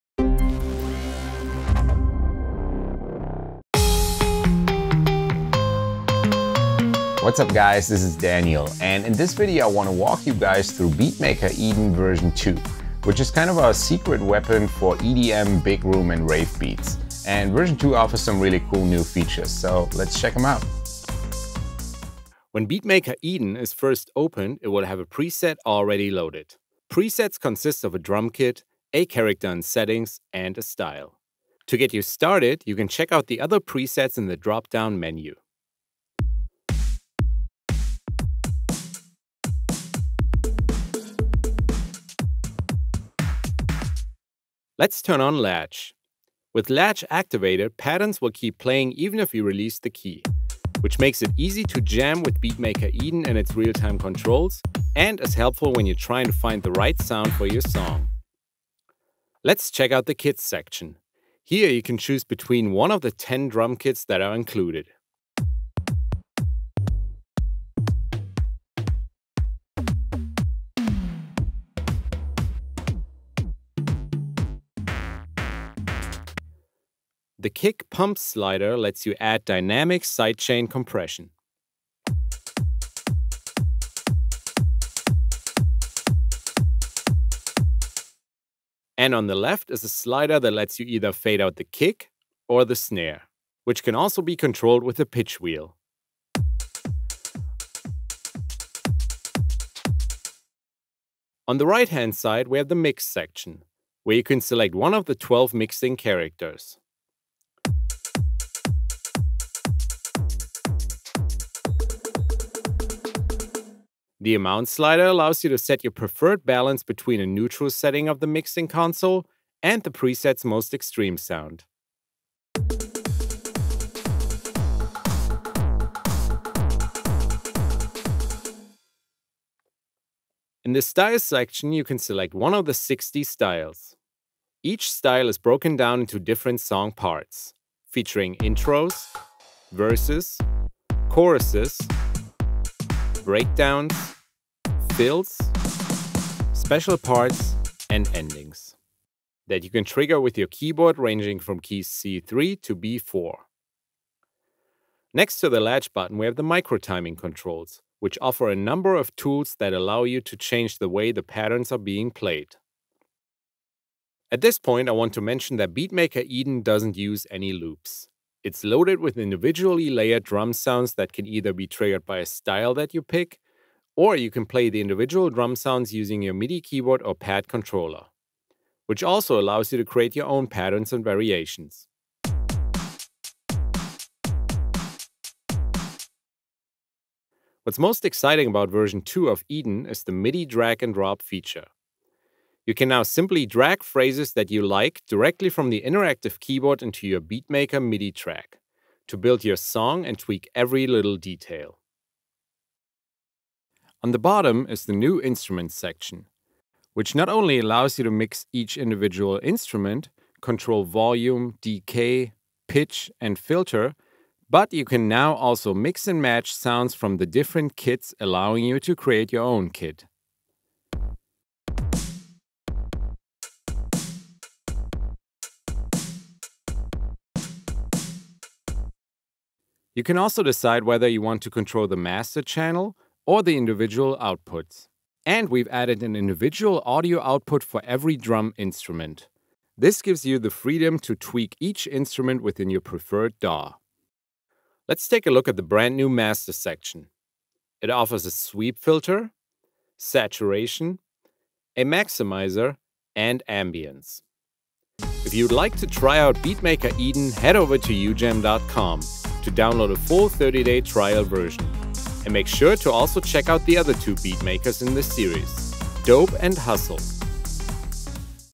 Walkthrough | Beatmaker EDEN 2
Let Daniel take you on a tour through our dance music and EDM specialist Beatmaker EDEN 2. It is your tool for building the right foundation for the next big summer anthem or four to the floor main-room monster.

He takes a brief look at each section of the user interface, giving you a head start for your next production. Here‘s what you’ll learn today:

0:22 Presets
0:44 Latch
0:59 Drum Kits
1:18 Signature Control: Kick Pump
1:29 Kick/Snare Fader
1:42 Mix Section
1:54 Amount Slider
2:11 Styles
2:35 Micro Timing
3:10 MIDI Drag and Drop
3:24 Instrument Section and Multiple Audio Outputs
4:06 Master Section
4:16 Free Trial